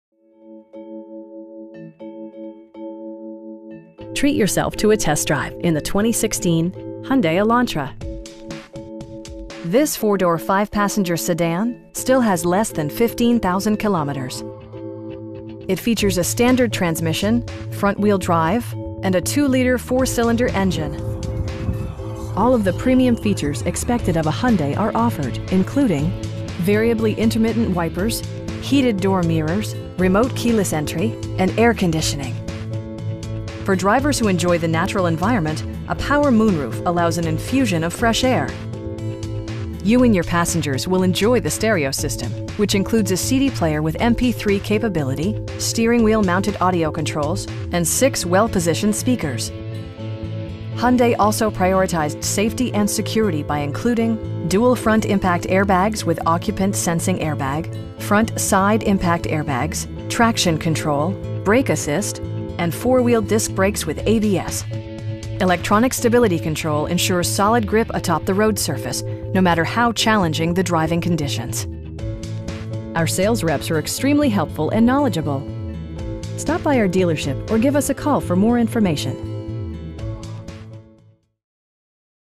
2016 Elantra GLS|401 Dixie Hyundai Walk-around
This 2016 Elantra GLS is coming back from its original owner. YES! a ONE OWNER vehicle that is looking for a new home.
The Elantra makes a styling statement, it is efficient, with a 1.8-liter inline-4 standard. Rated at 148 horsepower and 131 pound-feet of torque, this power-train is fairly smooth and accelerates respectably through a 6-speed manual transmission.
You are going to enjoy a Smart list of standard features like a back-up camera, heated seats, stylish alloy wheels, hands-free Bluetooth, power sunroof, Wireless phone connectivity, AM/FM Radio, Cd player and much more.